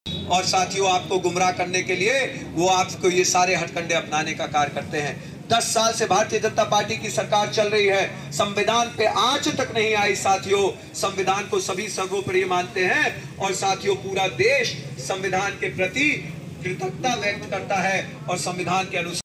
दस वर्ष से भाजपा की सरकार चल रही है..संविधान पर आँच तक नहीं आई है :भाजपा प्रत्याशी रितेश पाण्डेय
अम्बेडकर नगर...
✍️भीषण गर्मी के बीच भाजपा प्रत्याशी सांसद रितेश पाण्डेय द्वारा चौपाल का आयोजन..
✍️टांडा कश्मीरिया में वीर कमल के आवास के निकट जन चौपाल का आयोजन..
✍️ लोगों को संबोधित करते हुए कहा की 10 वर्ष से सरकार चल रही है..
✍️ संविधान पर आँच तक नहीं आई है,संविधान को सभी लोग सर्वोपरि मानते हैं..